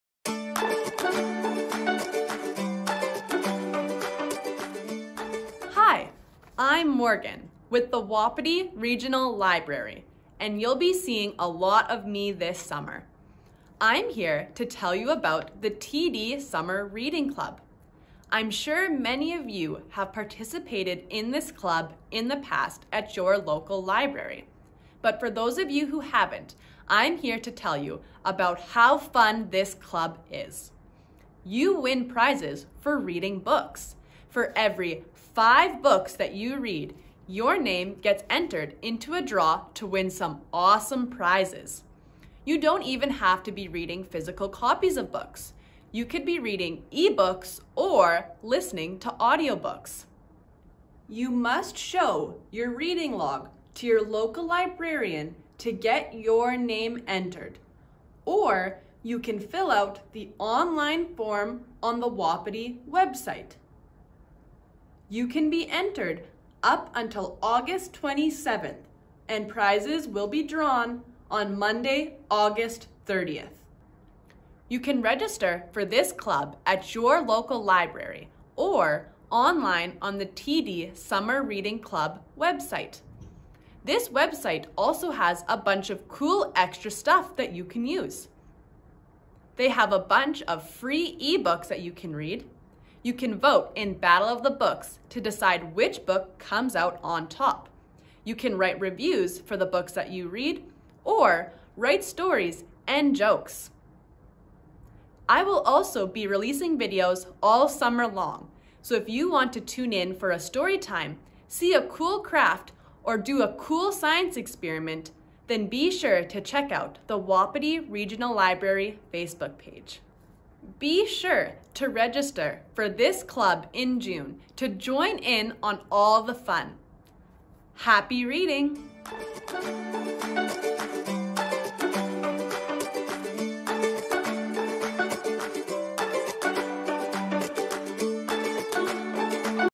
TD Summer Reading Club Introduction
Learn about the TD Summer Reading Club and the Wapiti Regional Library's summer program.

wapitilibrary.ca/SummerReading
tdsummerreadingclub.ca/about_the_club